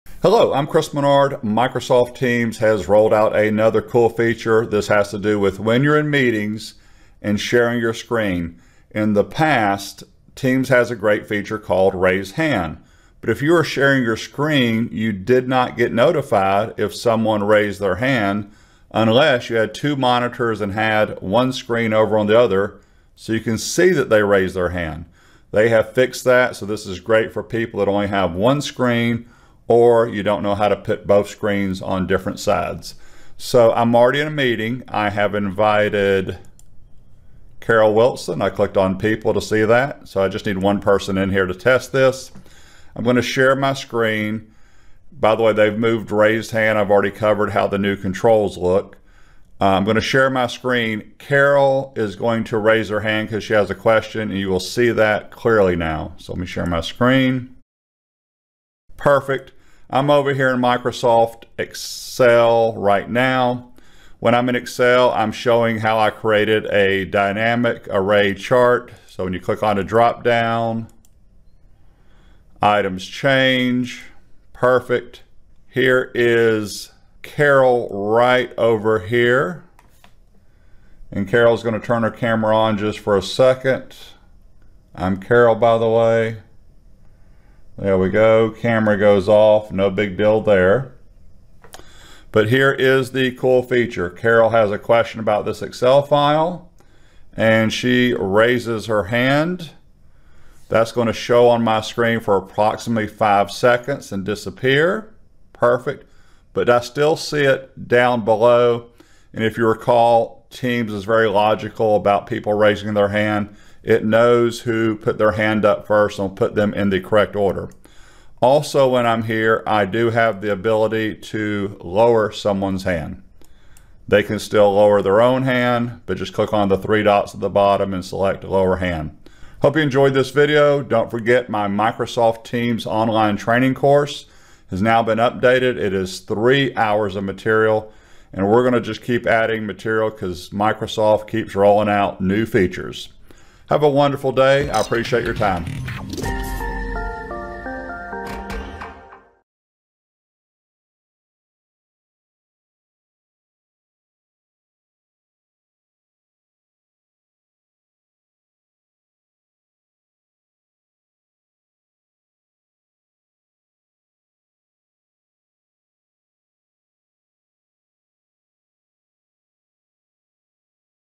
Teams improvement to screen sharing
Teams now allows the person sharing their screen to see raised hands. This is a great improvement. In the past, if a user raised their hand, the person presenting would not be notified. Also, the meeting controls when sharing is now at the top. They were located at the bottom.

Chapters:
0:00 Intro
0:40 Share Screen
1:28 Raised Hand
1:58 Lower a raised hand

-- EQUIPMENT USED
○ My camera